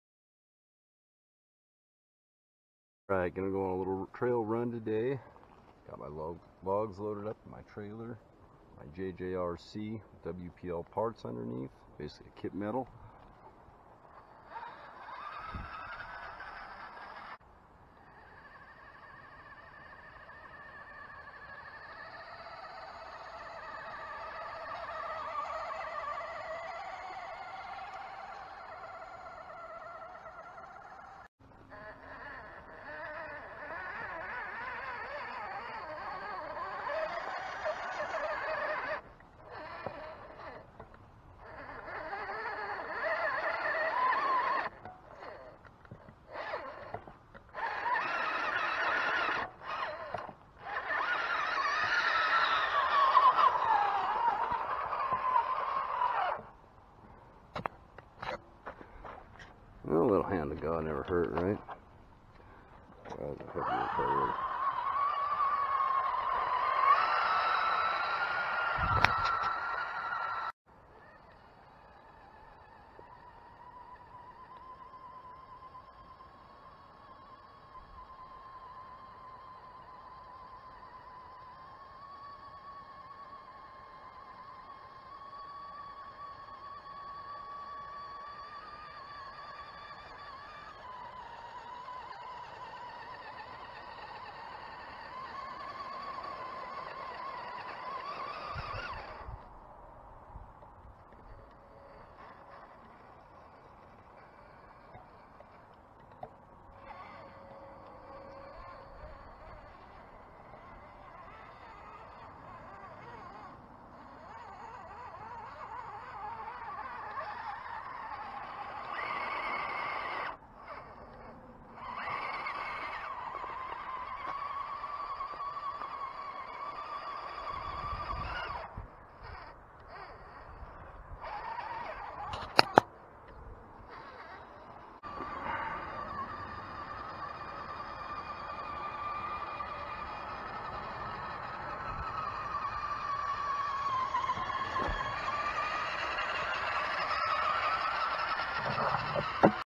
WPL JJRC 1:16 Zil-157 RC logging the Continental Divide with trailer overloaded.
JJRC 6x6 using WPL Kit Metal suspension, axles, and 2speed transmission. Hobbywing 1060 brushed ESC on 3s lipo battery.